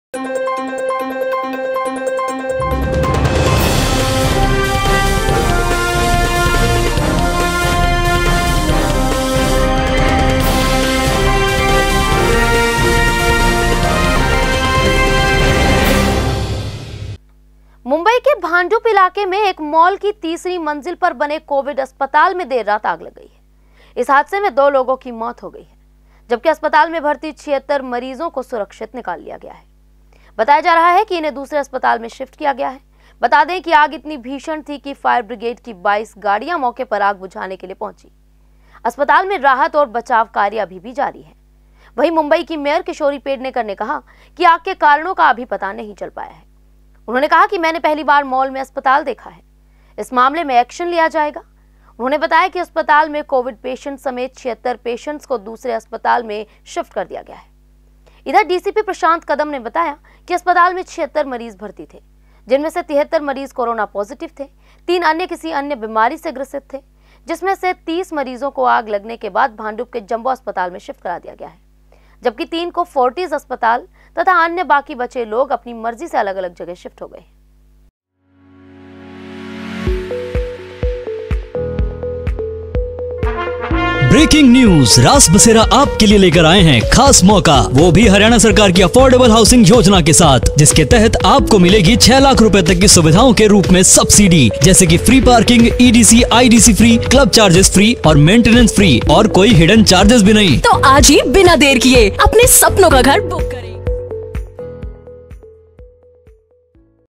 मुंबई में कोविड हॉस्पिटल में लगी भीषण आग
मुंबई के भांडुप इलाके में एक मॉल की तीसरी मंजिल पर बने कोविड अस्पताल में देर रात आग लग गई है। इस हादसे में दो लोगों की मौत हो गई है, जबकि अस्पताल में भर्ती 76 मरीजों को सुरक्षित निकाल लिया गया है। बताया जा रहा है कि  इन्हें दूसरे अस्पताल में शिफ्ट किया गया है। बता दें कि आग इतनी भीषण थी कि फायर ब्रिगेड की 22 गाडिय़ां मौके पर आग बुझाने के लिए पहुंची।